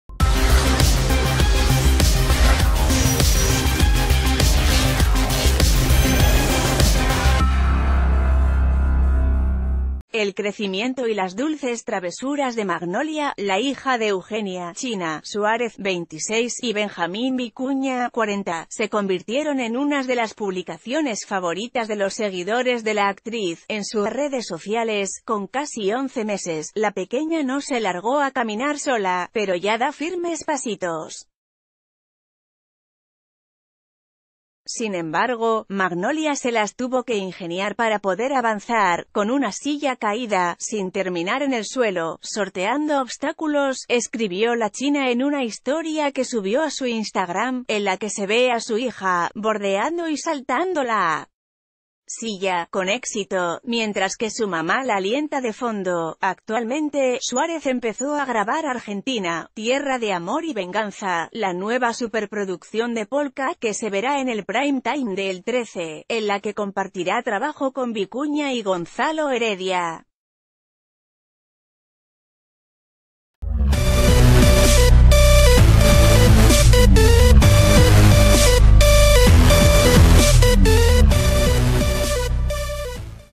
El tierno video de Magnolia, la hija de la China Suárez y Benjamín Vicuña, caminando con "obstácu...
El tierno video de Magnolia, la hija de la China Suárez y Benjamín Vicuña, caminando con "obstáculos"

El crecimiento y las dulces travesuras de Magnolia, la hija de Eugenia "China" Suárez (26) y Benjamín Vicuña (40), se convirtieron en unas de las publicaciones favoritas de los seguidores de la actriz, en sus redes sociales.  Con casi once meses, la pequeña no se largó a caminar sola, pero ya da fir...